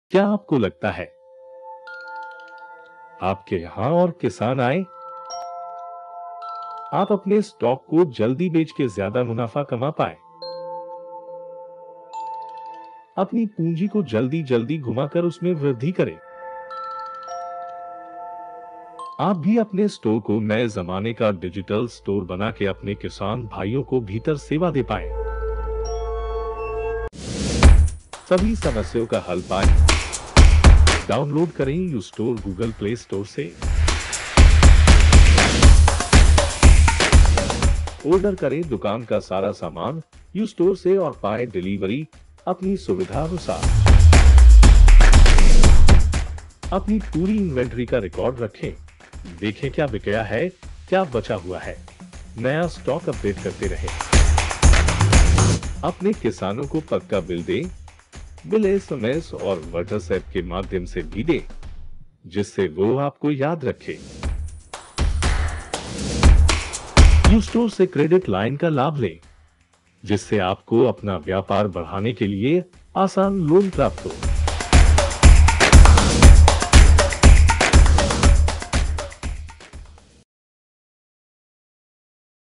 uStore (Your Digital Agri Store)
One-stop for all agricultural-related needs. Convert your store in a Digital Store.
Order Agri Inputs and Get delivery at your doorstep
Manage your Inventory and keep your stock updated
Give invoices to your farmers and Keep track of your accounts
Get a Credit line. Take a business loan to expand your business.
many more mind-blowing features. Download uStore Now and start ordering.